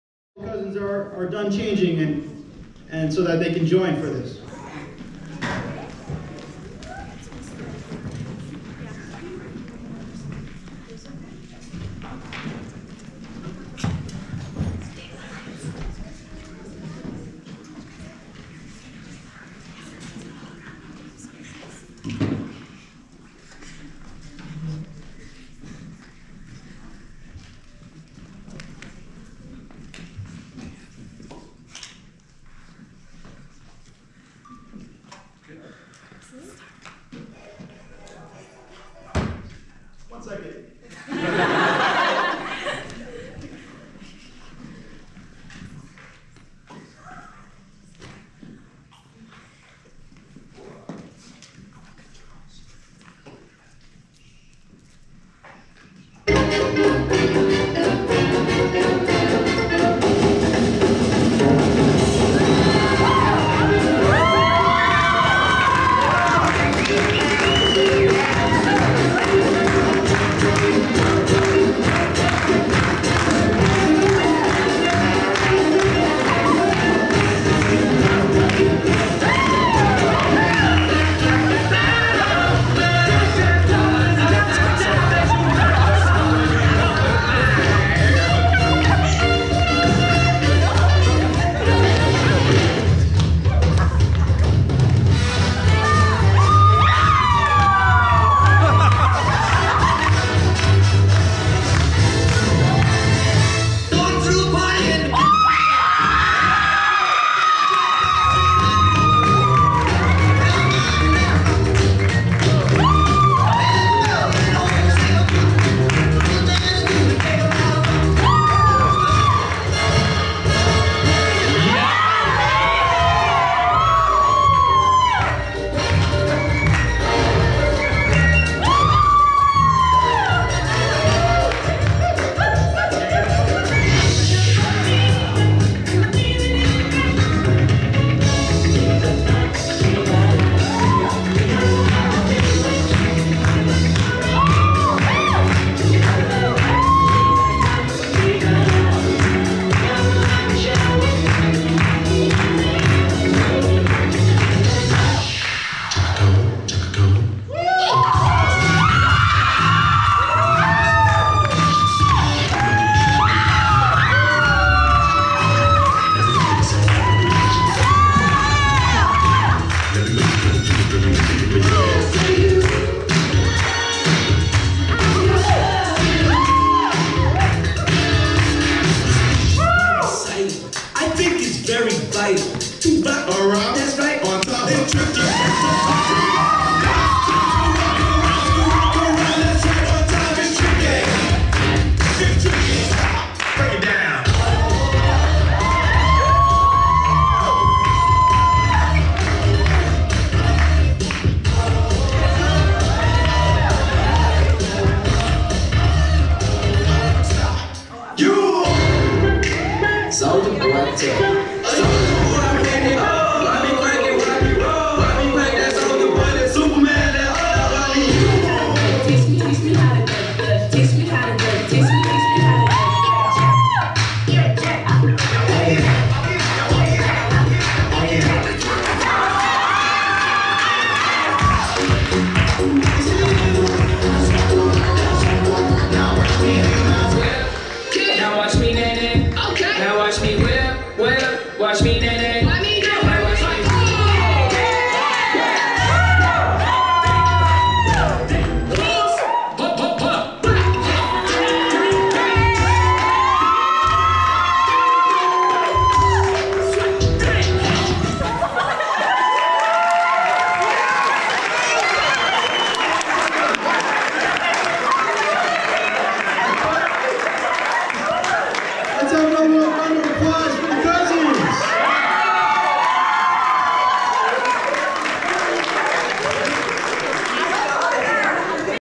Cousins Dance - "Dance thorough the Ages" VR 360 - Fiesta de Quinceañera de Isabella.
Isabella and the cousins prepared a surprise fun special number during the fiesta de Quinceañera.
Dancing the "Evolution of Dance" displays each decade and it's unique dancing style. This was a Fiesta every time they got together to practice!.